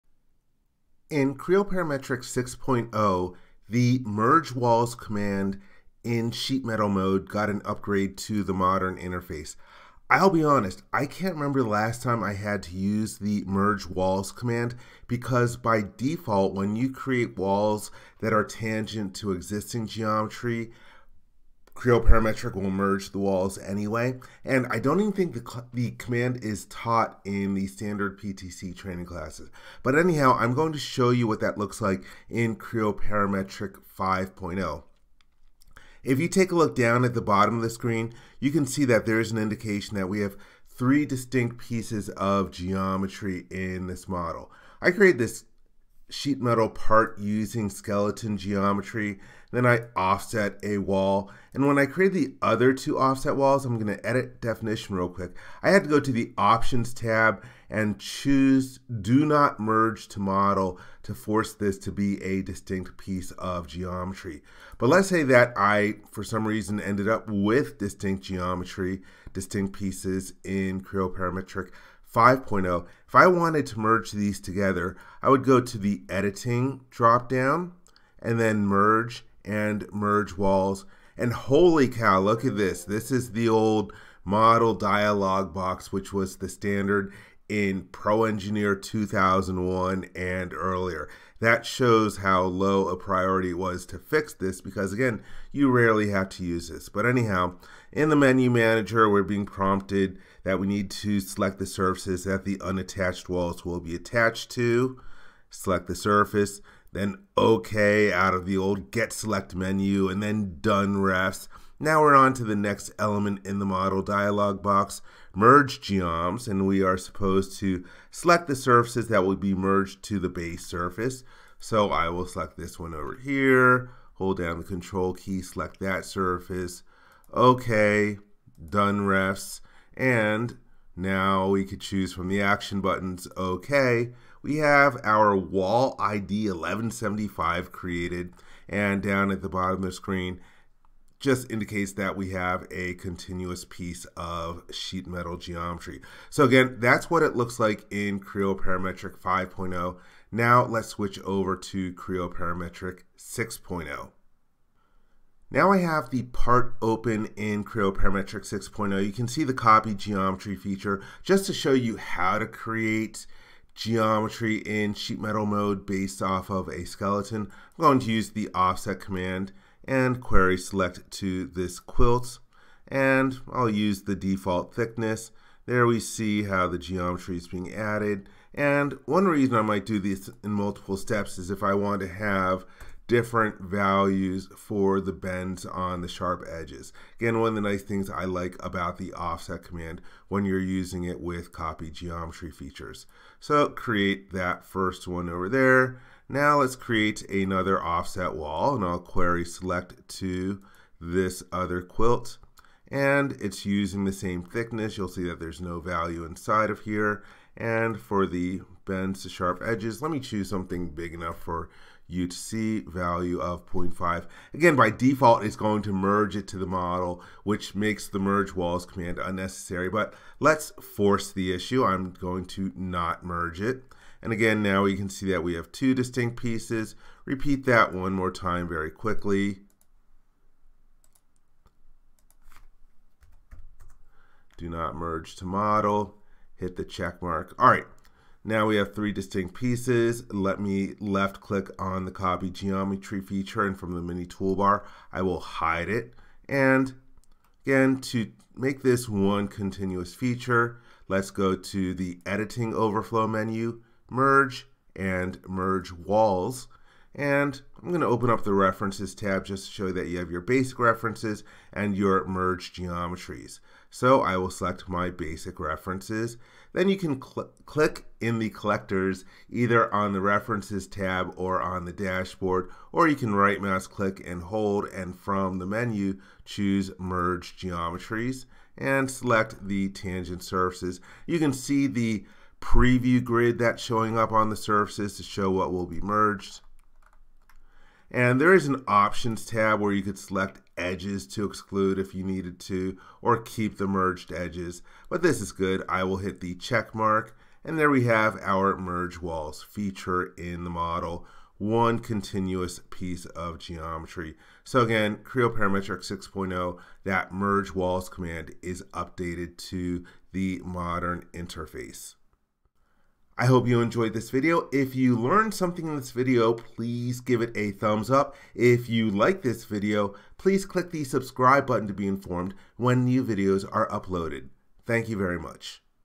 Creo Parametric 6.0 - Sheetmetal Merge Walls
This Creo Parametric 6.0 tutorial video shows how the Merge Walls command in Sheetmetal mode was updated to the modern interface.

Thanks,
Dave